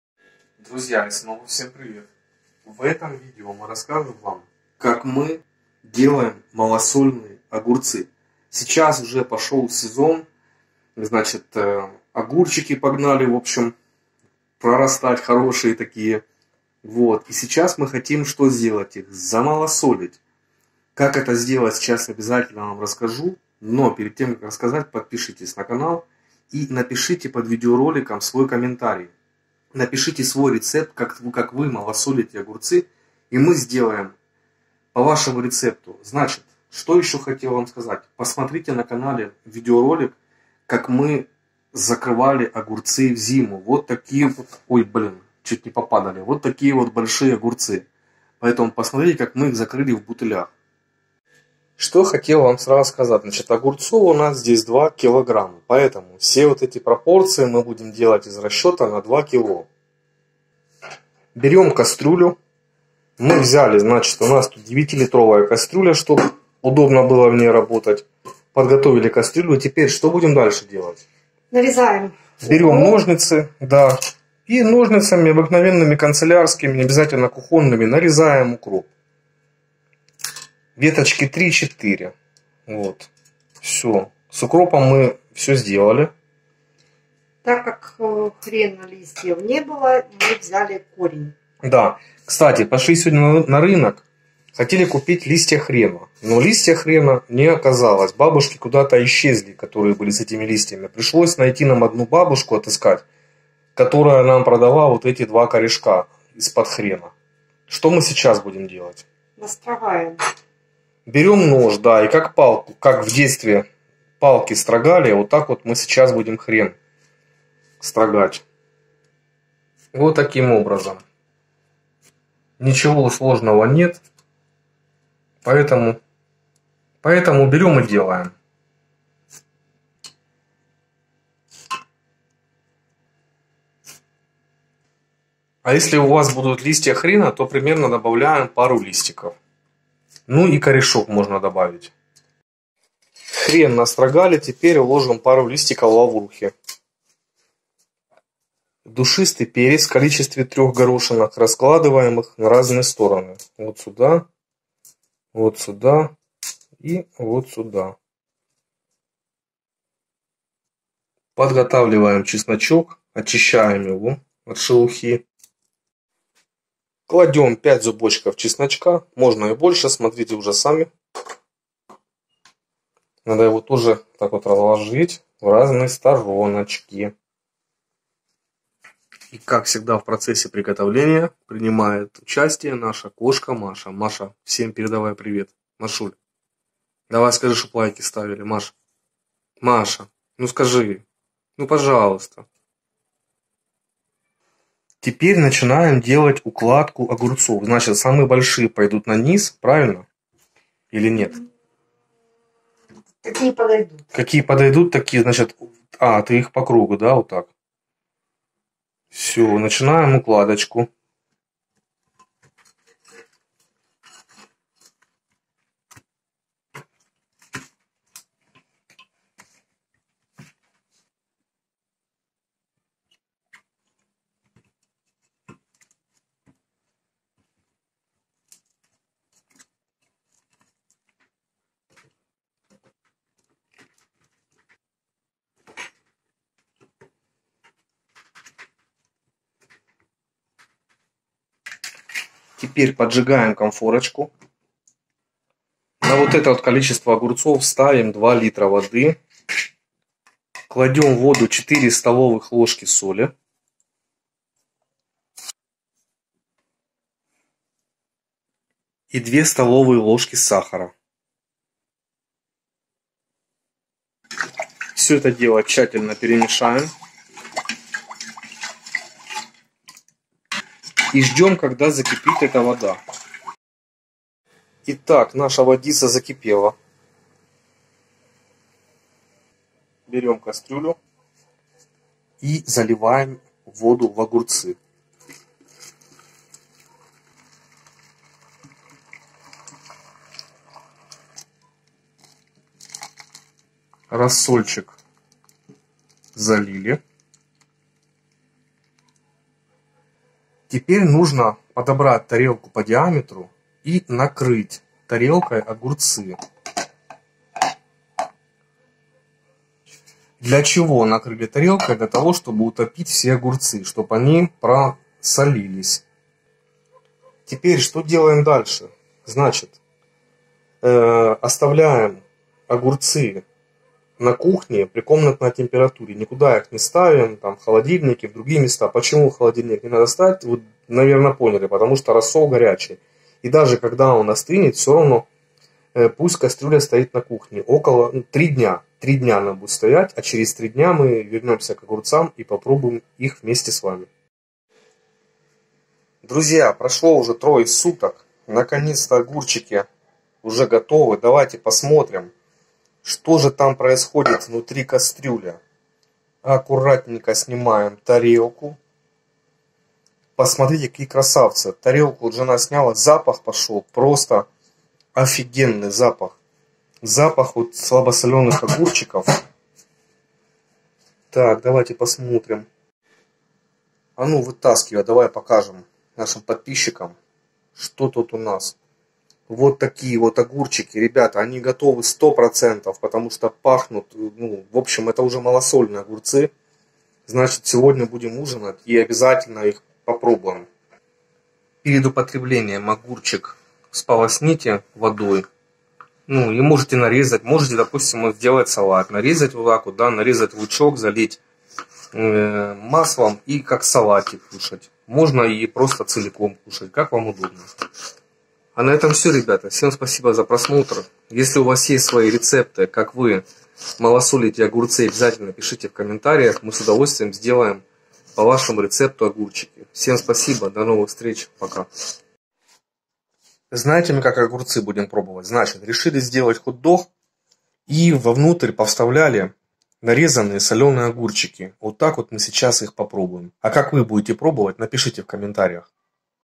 Бочковые огурцы в банке рецепт приготовления малосольных огурцов в кастрюле
Бочковые огурцы в банке рецепт приготовления малосольных огурцов в кастрюле - как замалосолить огурцы ?
В этом видео ролике мы покажем вам как малосолить огурцы в кастрюле в домашних условиях. Процесс засолки огурцов занимает 3 суток, после этого времени огурчики получаются слабосоленые, можно и дольше их замалосаливать и огурцы подскиснут. Тут уже на любителя !
Как промолосолить огурцы ?
Мы купили 2 кг. огурцов и на этот вес будем писать рецептуру.
Кладем в кастрюлю нарезанный укроп (3-4 ветки), пару листов нарезанного хрена либо 2 корня хрена (строгаем его ножом как в детстве палки), далее кладем 5-6 зубочков чеснока, 3 горошины душистого перца и укладываем огурцы, Далее ставим другую кастрюлю с водой и доводим ее до кипения. В воду кладем 4 столовые ложки соли и 2 столовые ложки сахара. Далее кипящую воду наливаем в огурцы и накрываем сверху тарелкой и крышкой, даем огурцам просолится 3-е суток. Перед употреблением бочковые огурцы можно промыть водой. Всем приятного аппетита !

►►►НА РАЗВИТИЕ КАНАЛА (по возможности). Всем Спасибо !
ИСТОЧНИК НОВОСТИ : Обзорочка ТВ
ОЧЕНЬ ИНТЕРЕСНЫЙ САЙТ, РЕКОМЕНДУЮ ВСЕМ !